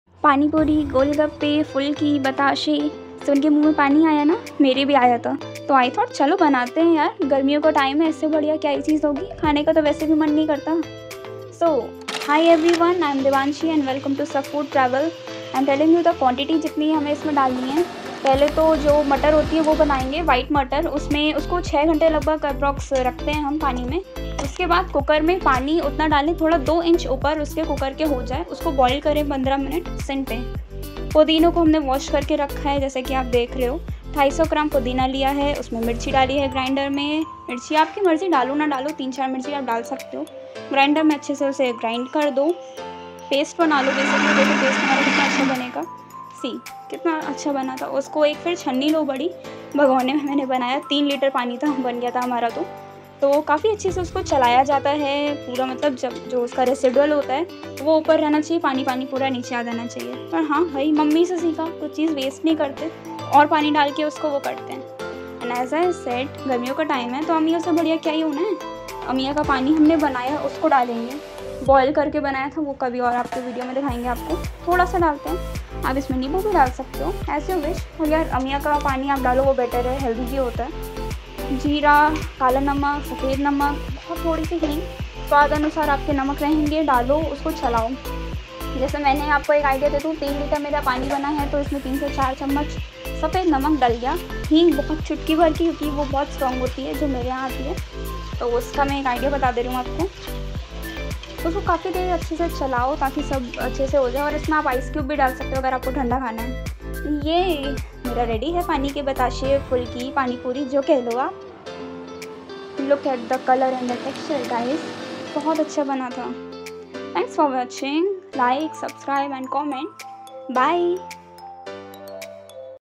Easy Masala Panipuri Recipe | पानीपुरी/गोलगप्पे | Panipuri ka Paani Recipe | Bilkul Bazar ki Tarah
Easy Homemade Panipuri Recipe

Follow The Instructions For Amazing Panipuri

Enjoy Your Homemade Delicacy With...